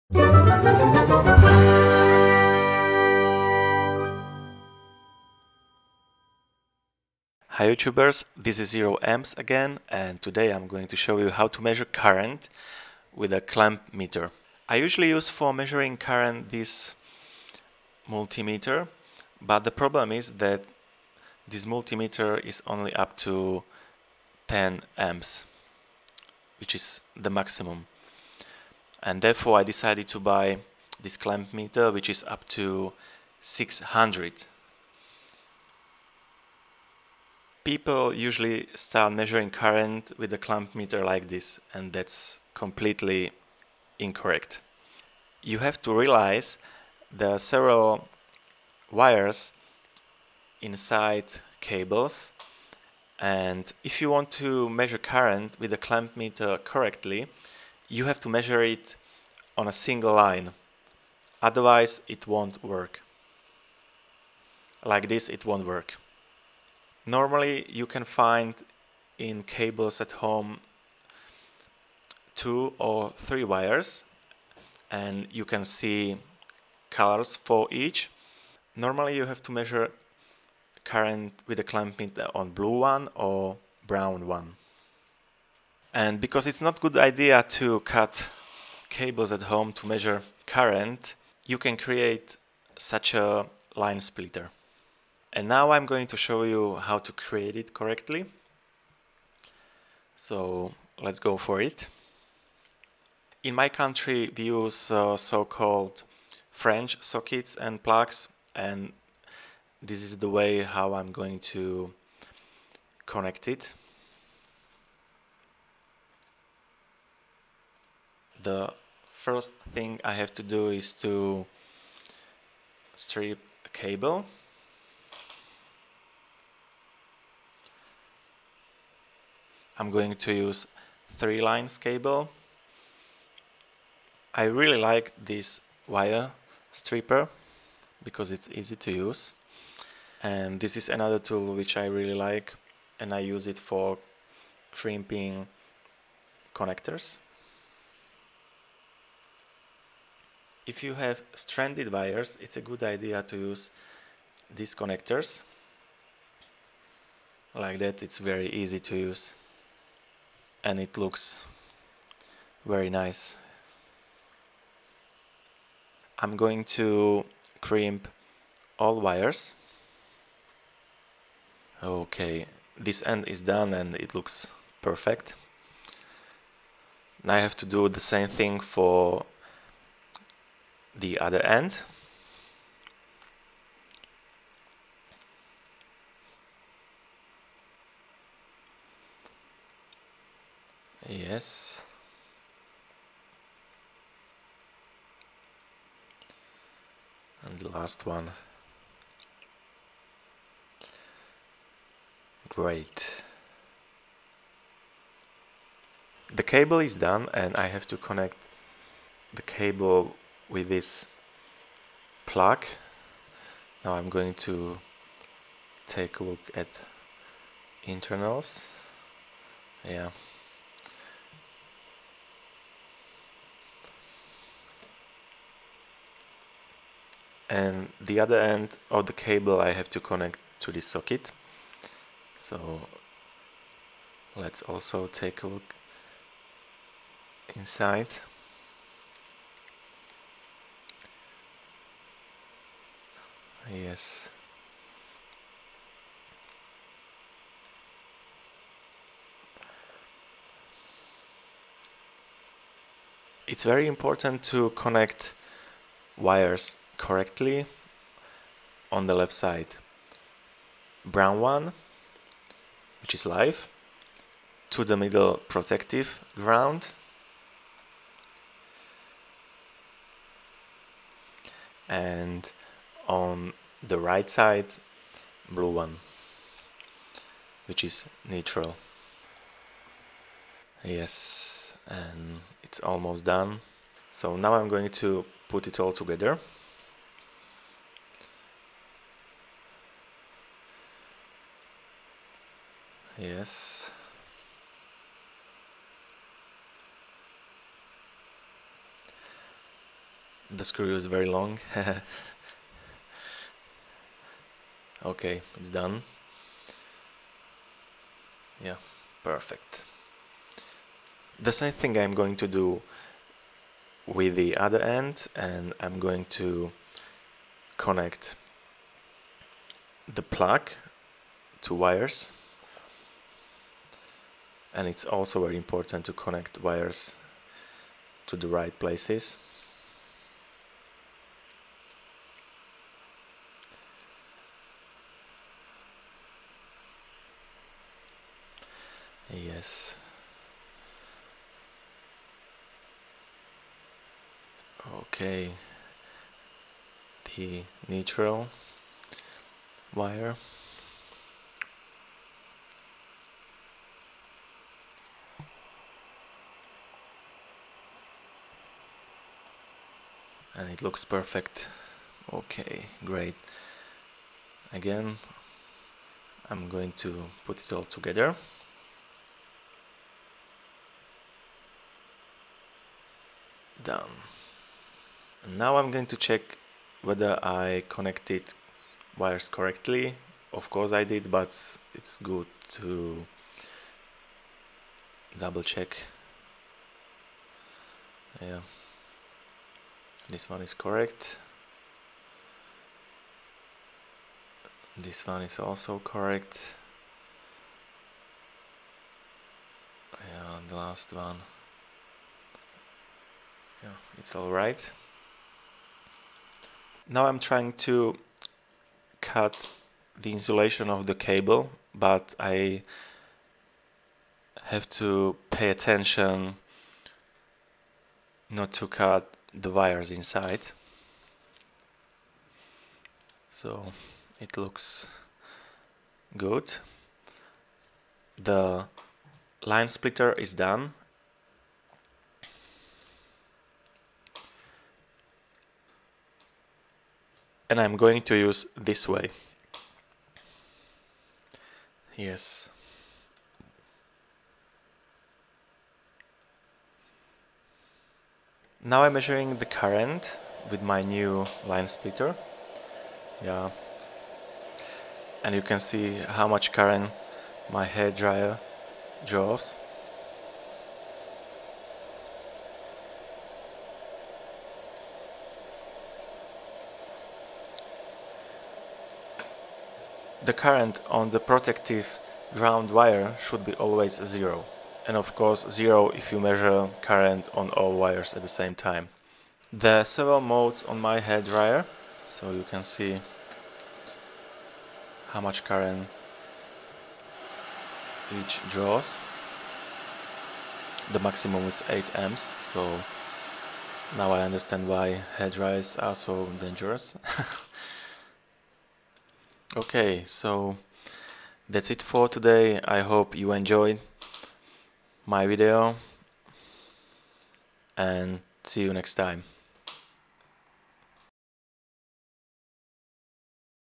How to Measure Current with Clamp Meter Using Line Splitter
Do you want to measure current with your clamp meter and you don’t know how to do that correctly? I show you how to use clamp meters and also step by step how to make a line splitter which makes measuring even easier. If you have any questions about measuring with clamp meters, ask us in comments.

🛠 Tools which I use in the video 🛠
➟ Multimeter UNI-T UT219DS
➟ Multimeter UNI-T UT61E
➟ Multimeter UNI-T UT120A
➟ Screwdrivers
➟ Wire Stripper
➟ Precision Knives
➟ Insulated Wire Connectors
➟ Crimp Tool

Stay creative and keep on tinkering 😊